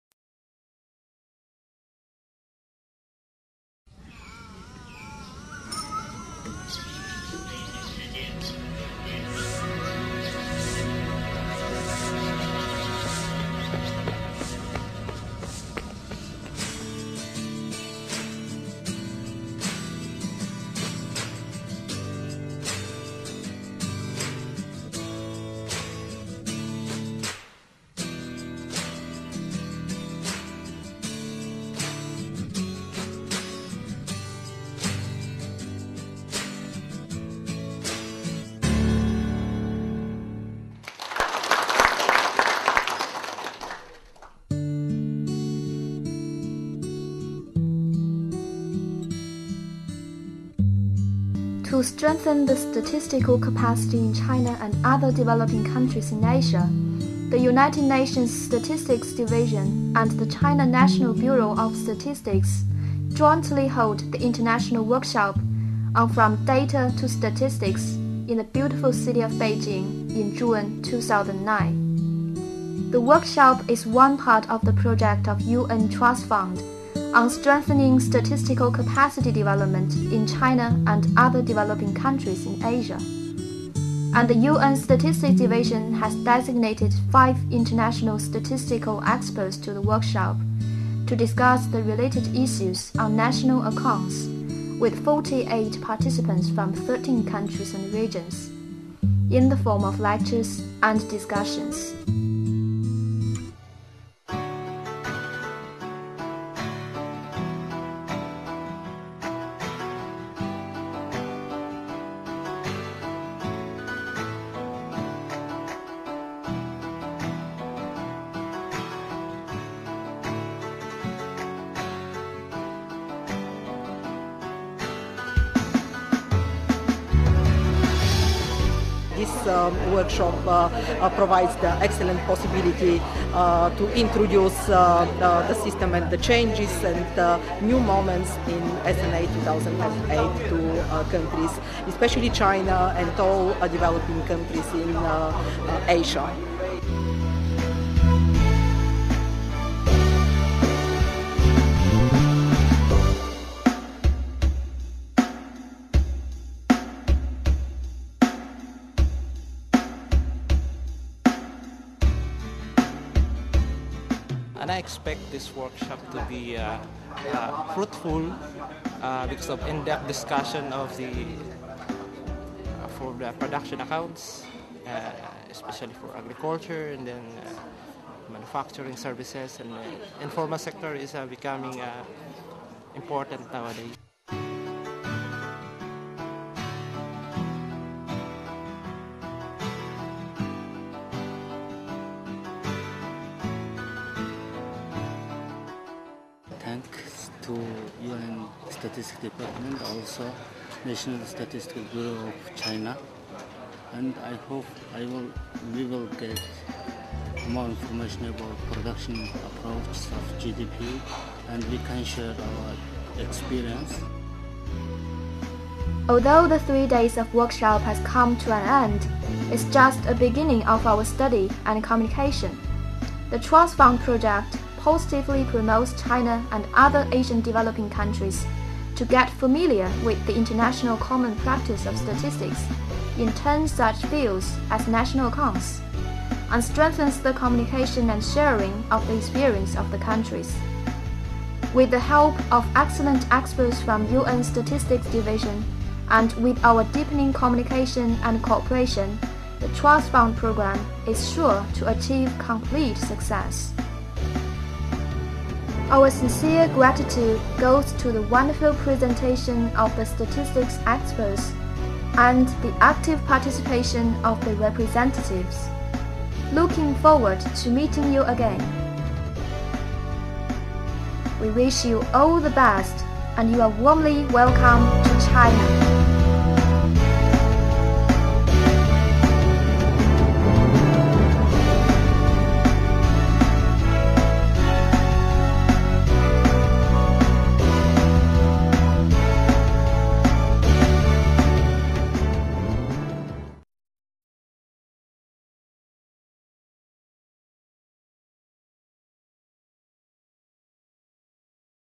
China 2009 - International Workshop "From Data to Accounts"
To strengthen the statistical capacity in China and other developing countries in Asia, the United Nations Statistical Division and the China National Bureau of Statistics jointly held the International Workshop "From Data to Accounts"